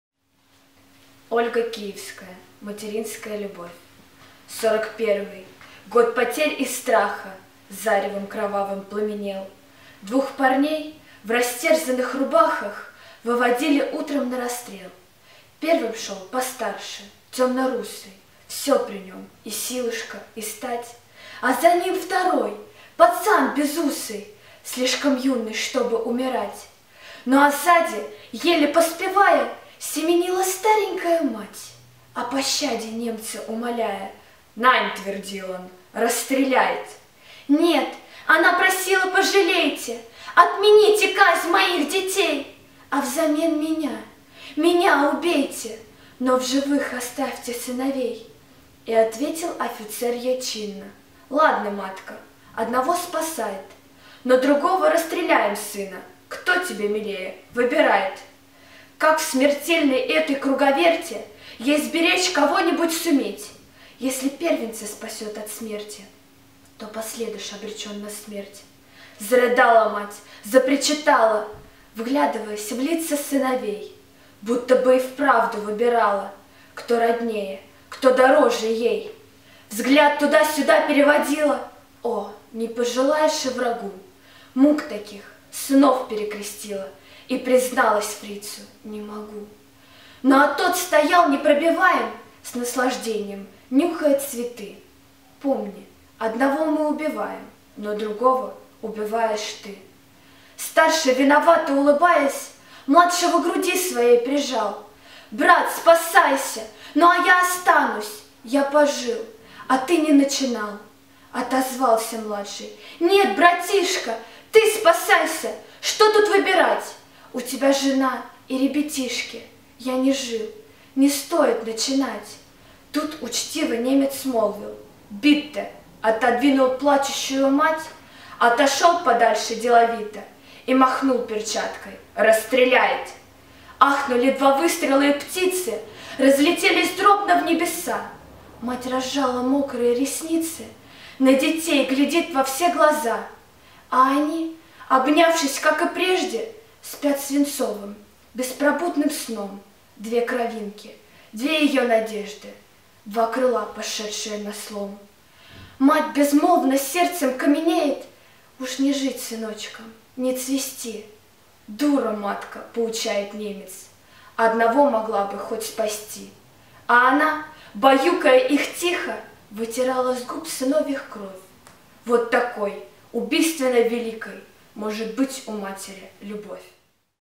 Ольга Киевская "Баллада о матери" (Ростов без наркотиков)
Ольга Киевская. "Баллада о матери". Читает воспитанница женского реабилитационного центра